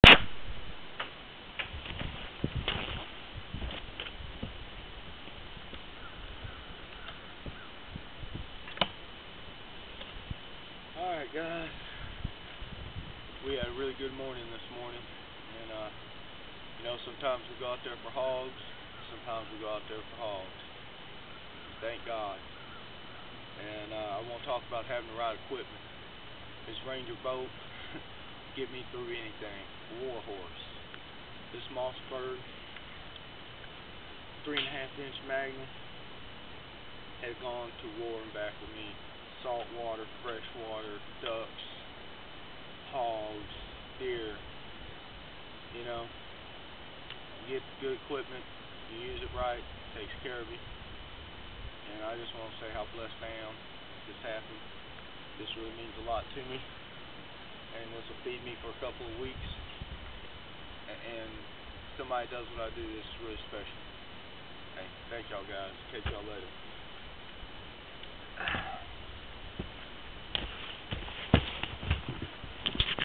Hog huntin at its best on Big Sam 2
One of th  Hogs I got that morning cause I did catch 2, 4 pound bass on ribbit frogs before I got to th spot where I anchored up. I sat there for no more than 10 min when the hogs started coming out onto the lake bed.